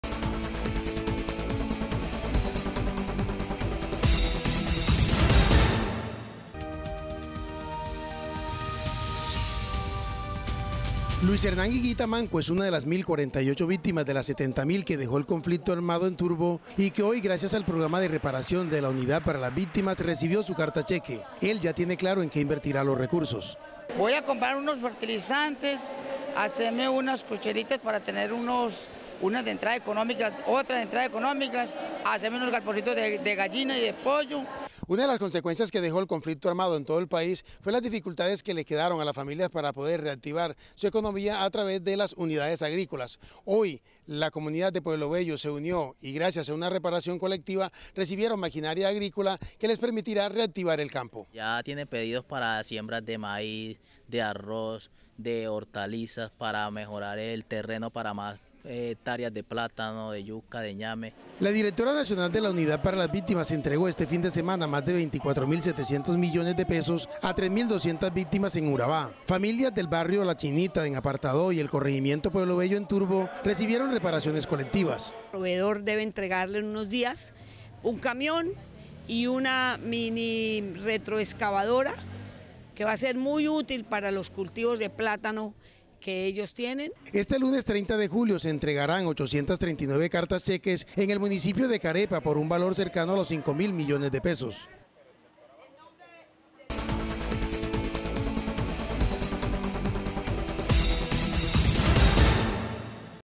Nuevas jornadas de reparación de víctimas en Urabá
Más de 20.000 millones de pesos fueron entregados por la unidad para las víctimas en Urabá, a cerca de 7.000 familias representados en indemnizaciones con carta cheques y reparaciones colectivas. En el corregimiento Pueblo Bello, Turbo, uno de los lugares emblemáticos de la violencia en Urabá, las familias recibieron estos apoyos.

-Teleantioquia Digital-